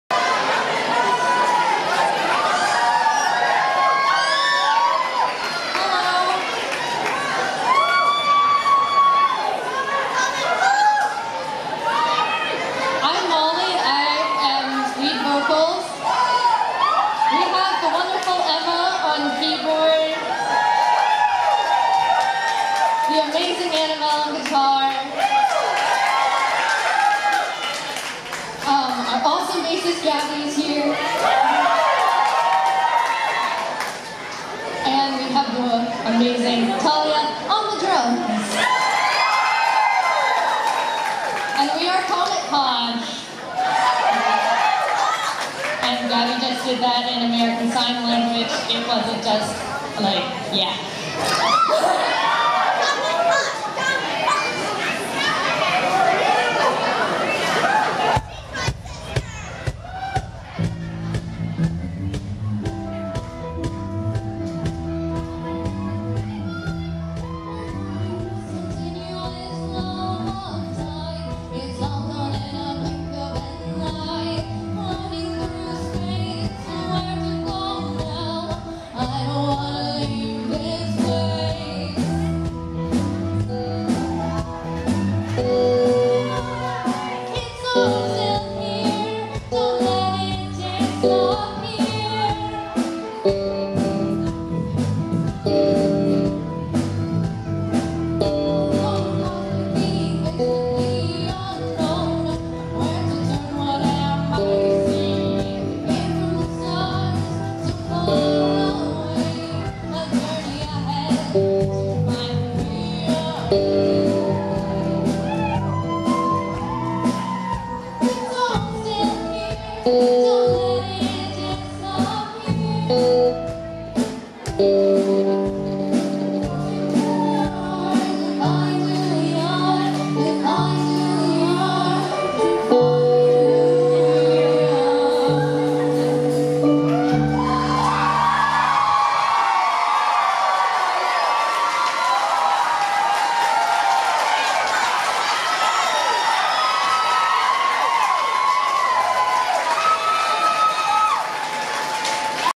Comet Posh - 2017 Showcase - Girls Rock Columbia
July 23, 2017 - Comet Posh performs at the Music Farm in Columbia, SC, in the Girls Rock Columbia 2017 Showcase.